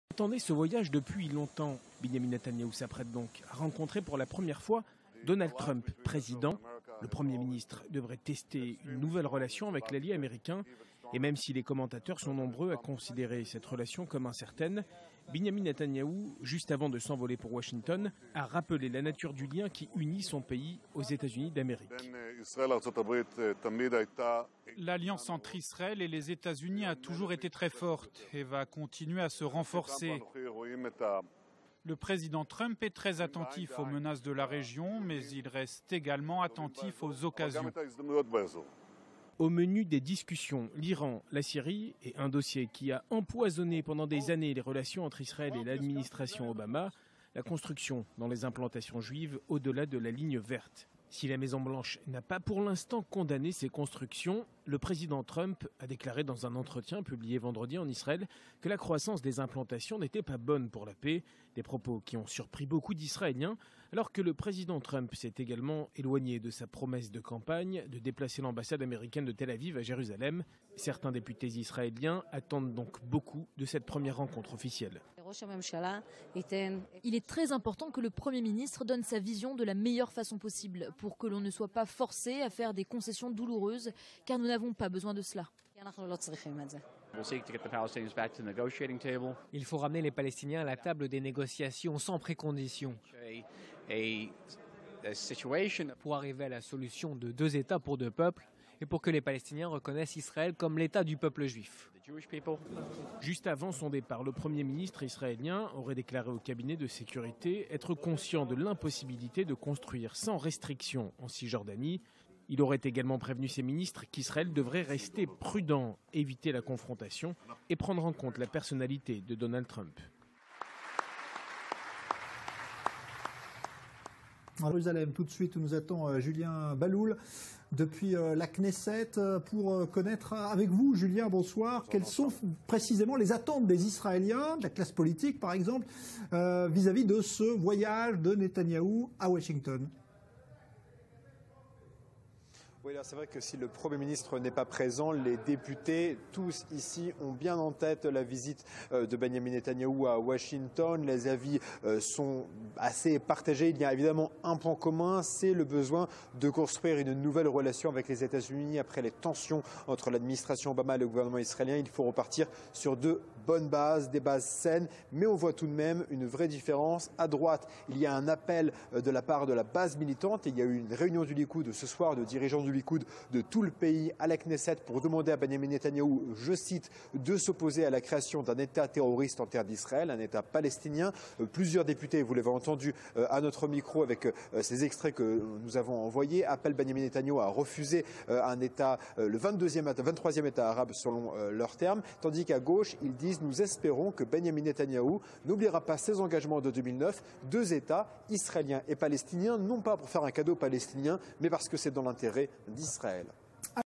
Netanyahou aux Etats-unis
Départ de Netanyahou aux Etats-unis pour rencontrer Donald Trump pour la première fois